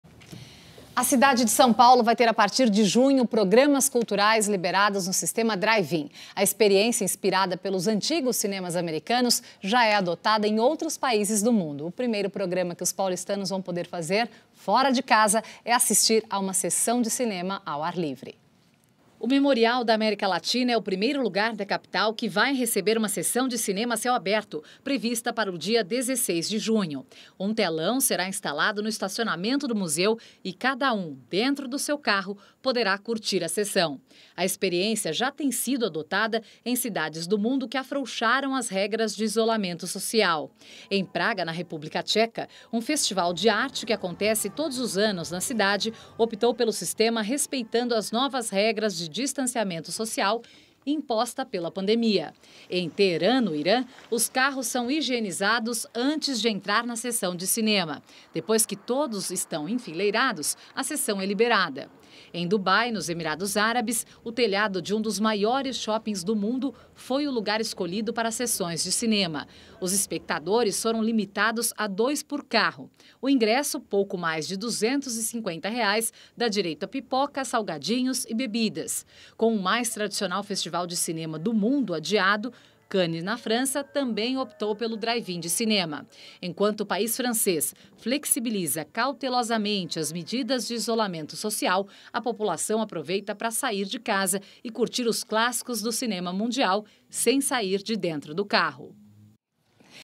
Cidade de São Paulo ganha programas culturais em 'drive-in'
A cidade de São Paulo vai ter, a partir de junho, programas culturais liberados no sistema "drive-in". A experiência, inspirada pelos antigos cinemas americanos, já é adotada em outros países do mundo. O primeiro programa que os paulistanos vão poder fazer fora de casa é assistir a uma sessão de cinema ao ar livre.